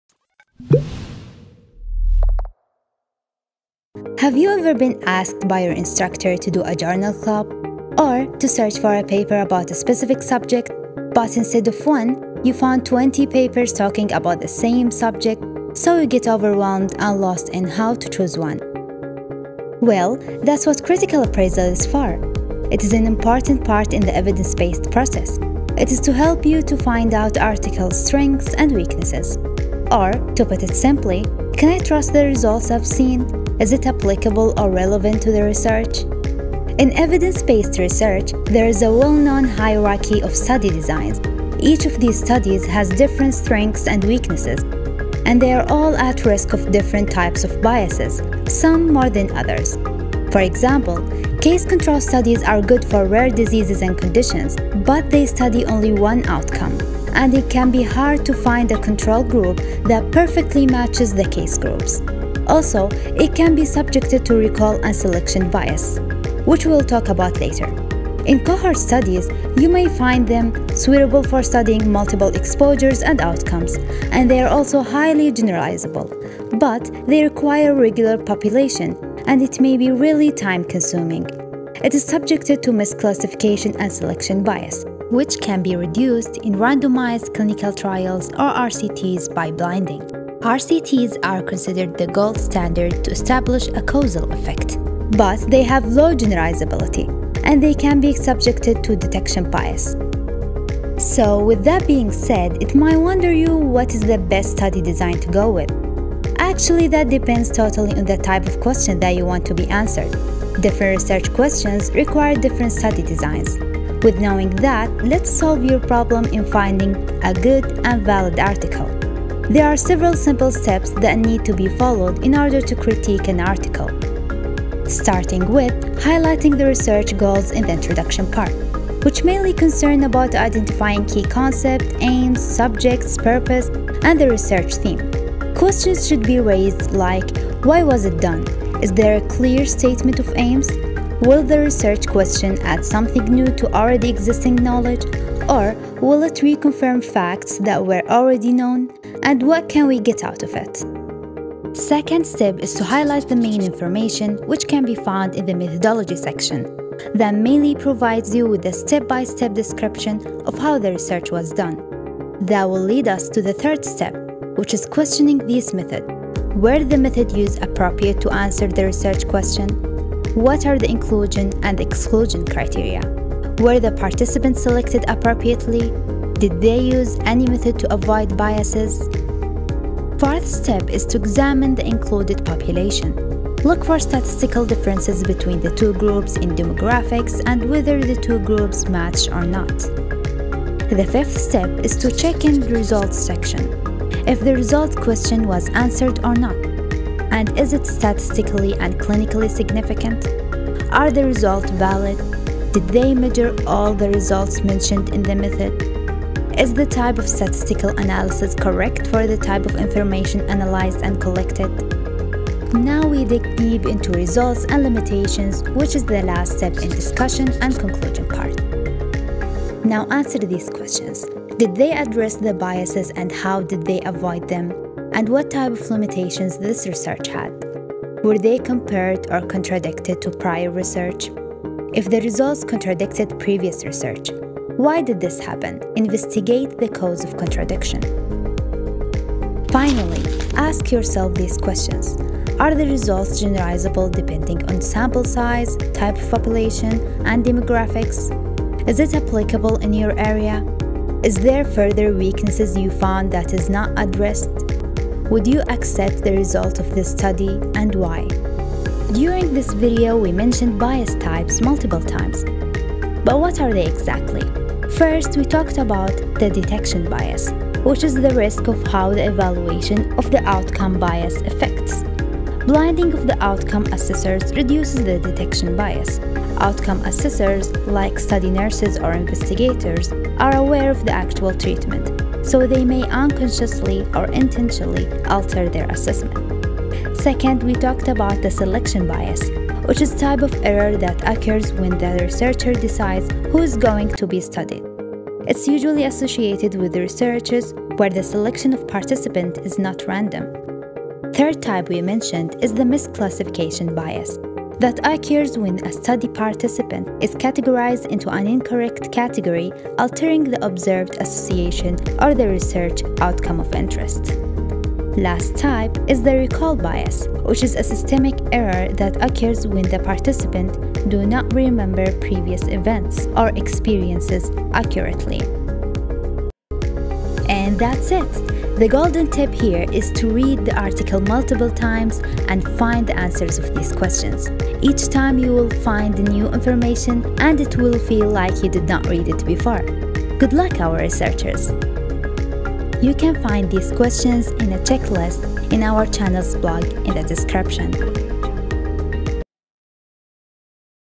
Critical Appraisal ! A way to assess a research paper better
Writing: Khuld Aloufi
Content Creator: Abeer Alturki
Voice Over and all managed by Sarah Alkanhal
Video Editor: Motionesta

Hello Everyone!
I hope you liked the video, if you did please share it with your colleagues and friends!

Special Thanks to Dr Ahmed Aljedai for reviewing the information in this video

this video is done with the collaboration and sponsorship of saudi society of clinical pharmacy